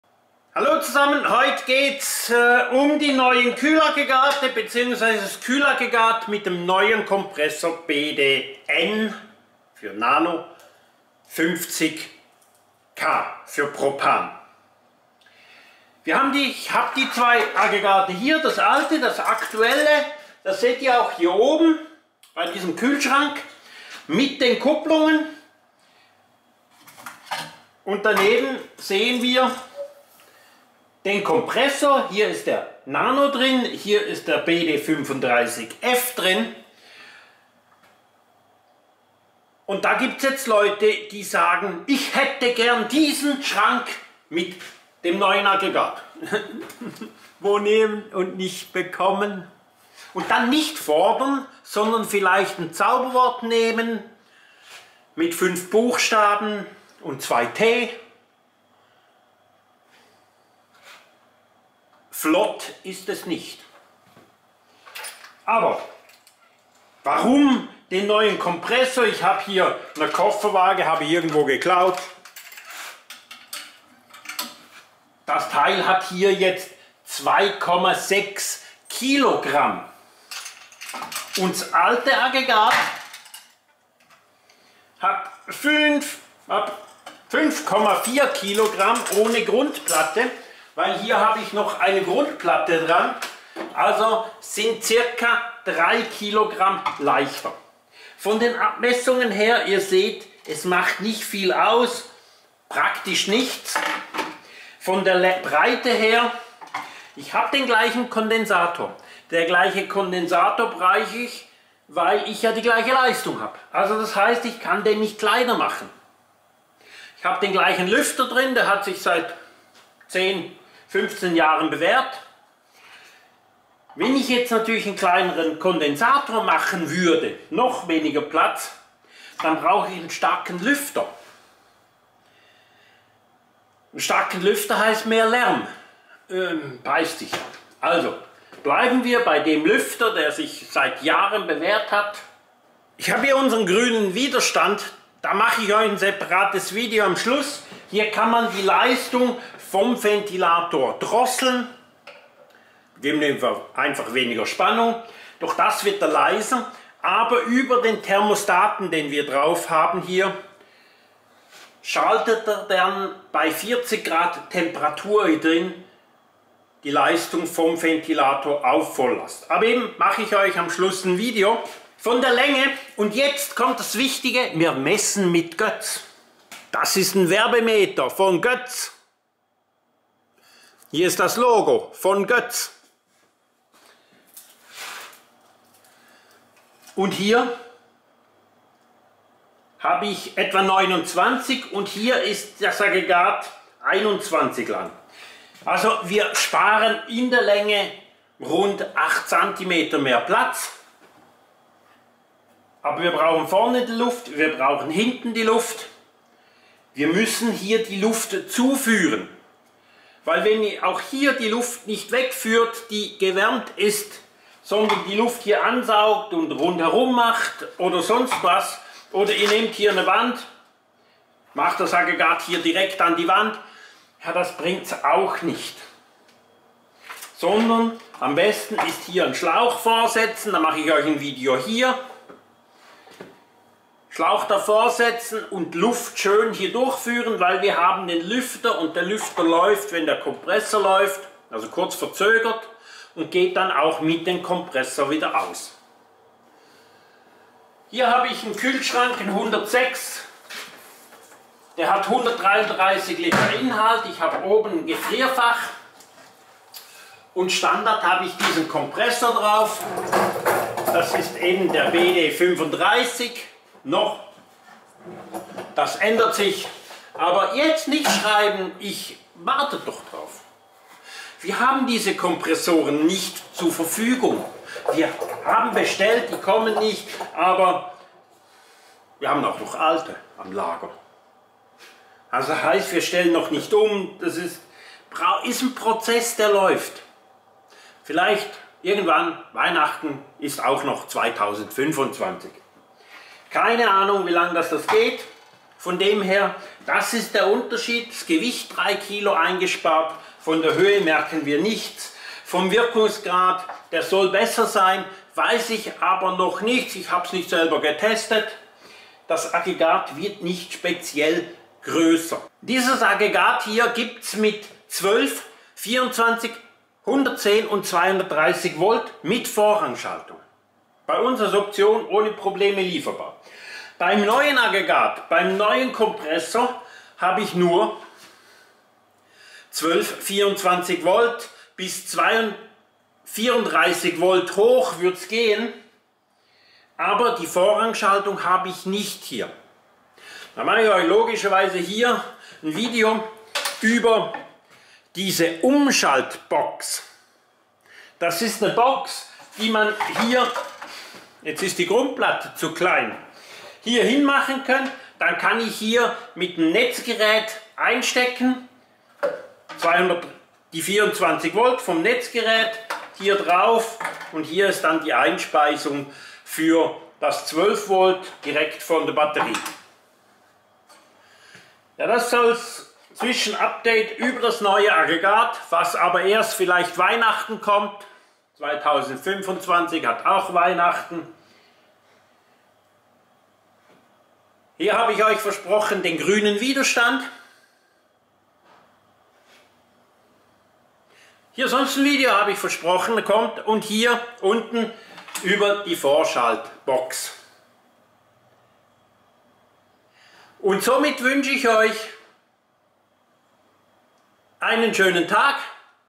Neues Kühlaggregat BDN50K Secop leichter kleiner 12 / 24 Volt Batteriestrom nano Aggregat Kupplung
neues Kühlaggregat

► KATALOGE:
Boot und Caravan Kühlgeräte 12 Volt  24 Volt 2024